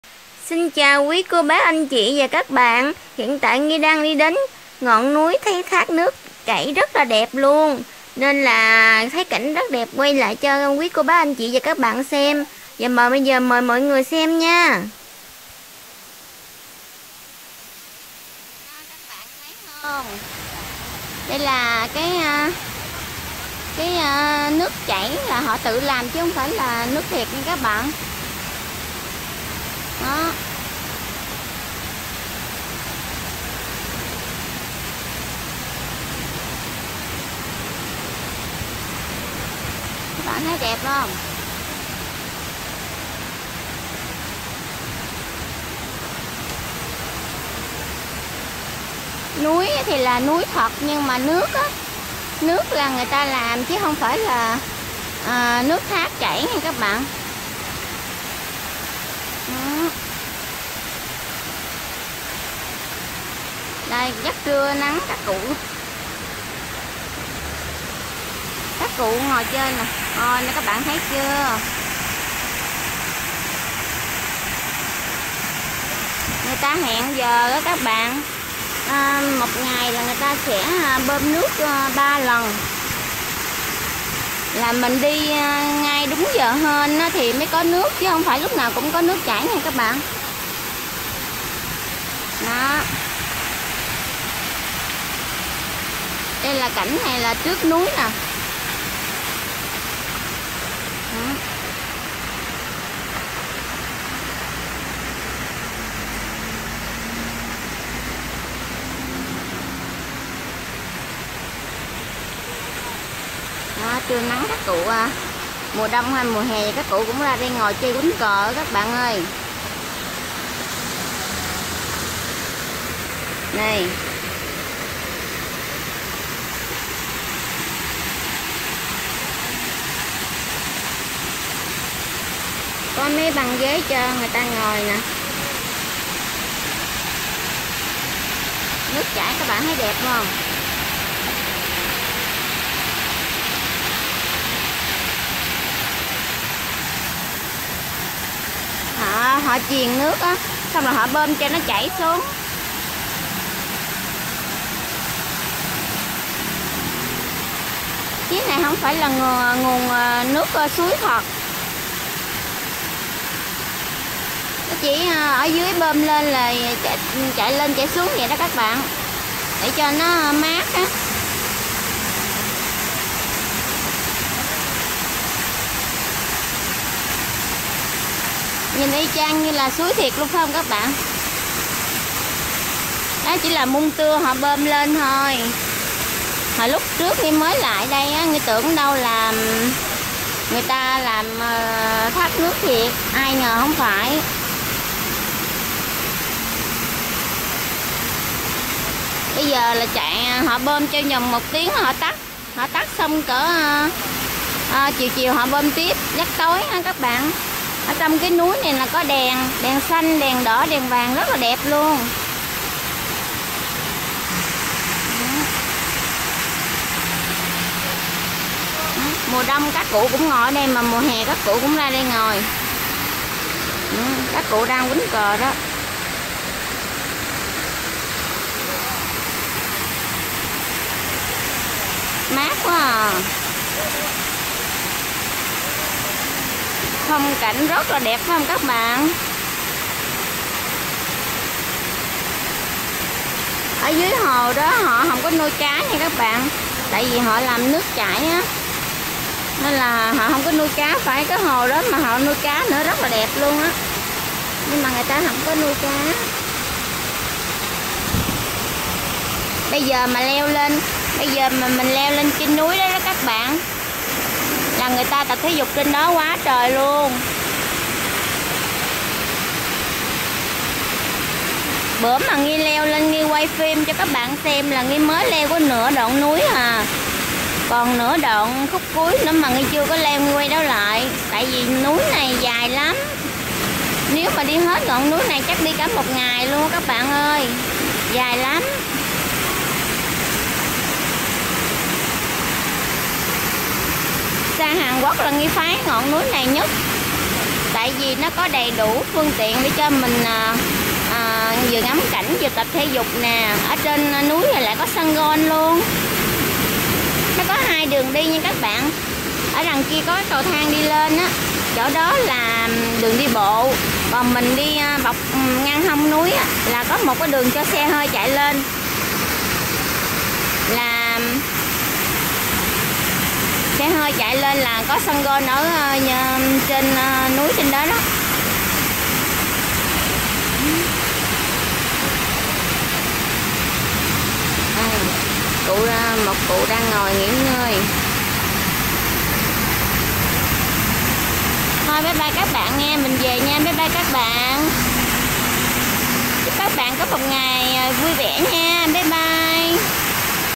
THÁC NƯỚC KÌ DIỆU Ở GIỮA THÀNH PHỐ KOREA(Hàn Quốc)//QUÁ XÁ ĐẸP 💥💖💥/NGHI NGUYEN CHANNEL
Thác nước kì diệu ở giữa thành phố korea Hàn Quốc.Một ngày thác nước này sẽ chảy ba lần trong ngày vào thời điểm nóng nhất. Mùa đông thì kg có nước chảy. Vì đây là Thác núi đá thật nhưng nguồn nước thì do máy bom lên ak.Ở ngay trung tâm thành phố nóng bức mà có được thác nước như vậy quá lý tưởng